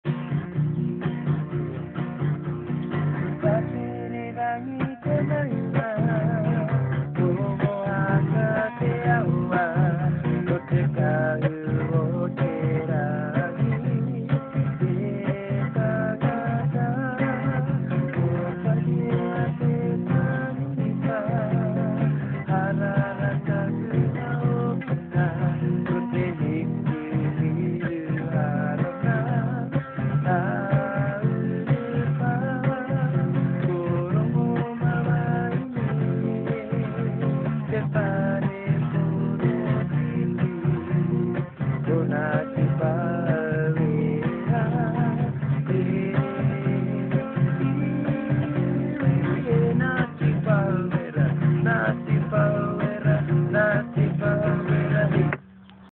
Ngati Pahauwera
Daniel singing at his nannies tangi.  neat there nephew.